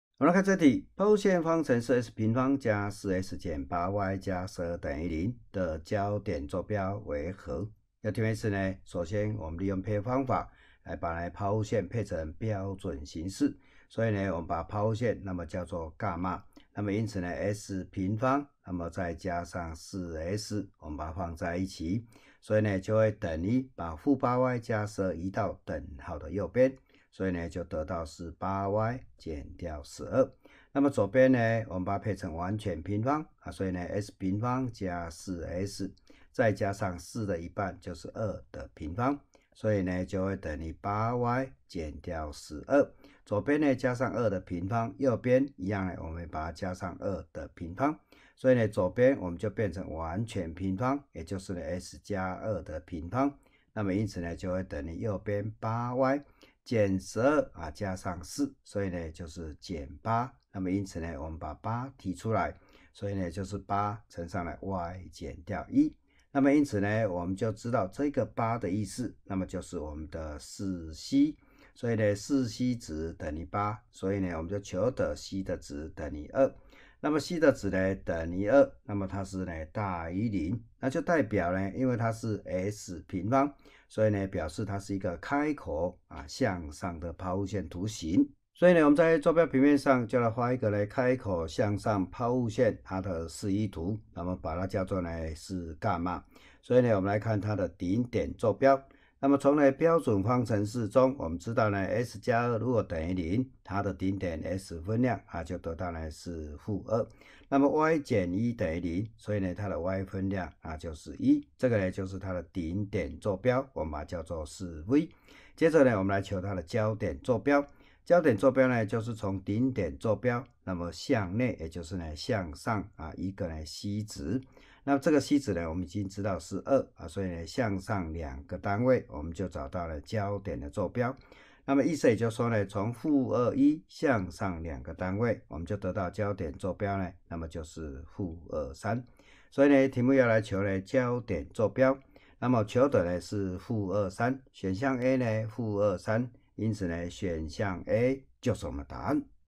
技高數學C (四) CH2 自我評量 第2題
由陳健在老師精心說明，
請參考技高數學C版第四冊課本P94。